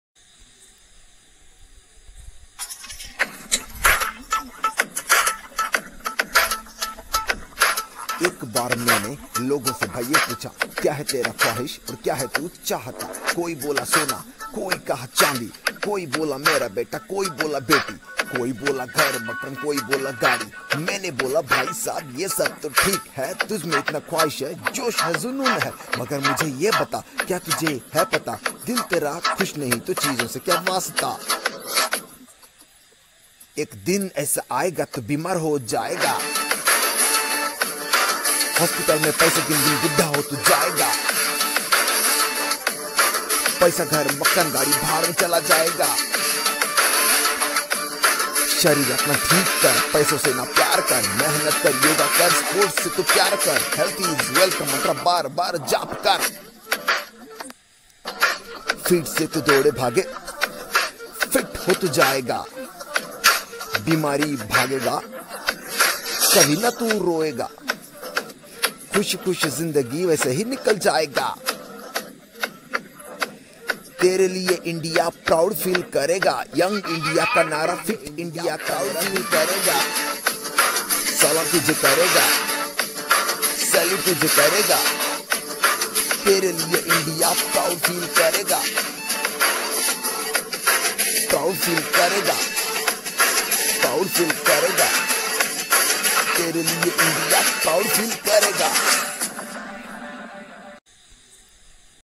PM Modi Launches Fit India Movement| 1st Rap Song based on it.
On 29th of August PM Narendra Modi launched "Fit India Movement" on the occasion of National Sports day vis-a-vis legend Major Dhyanchand's birthday. 🇮🇳

This is a small effort from my side to spread the awareness in the form of a rap song. This is my first attempt to compose and sing it.

I know it's not perfect but I have tried. 😋

Credit: The whole video editing has been done by my wife, Ritushree Nayak.😍